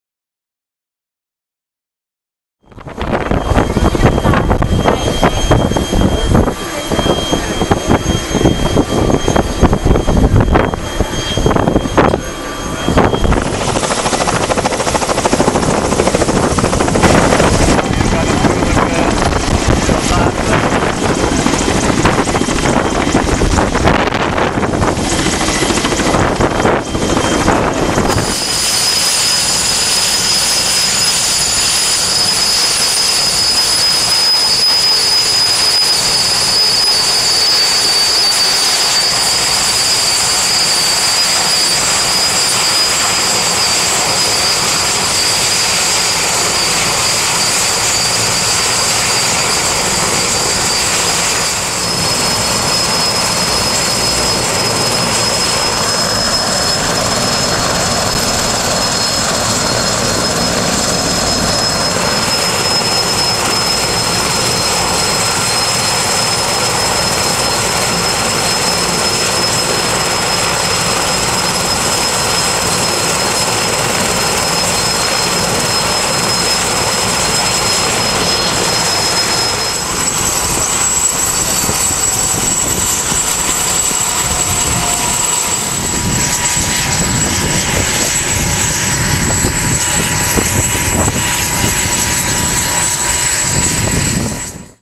Helicopters at Omiros Airport while refueling to continue to fight the fires in Chios.
Helicopters at Omiros Airport in Chios while refueling to continue to fight the fires.  The Russian pilots told The Greek News that the strong winds in the last 4 days made it very difficult to control them, and although they had to make an extra effort they were glad they had the chance to help.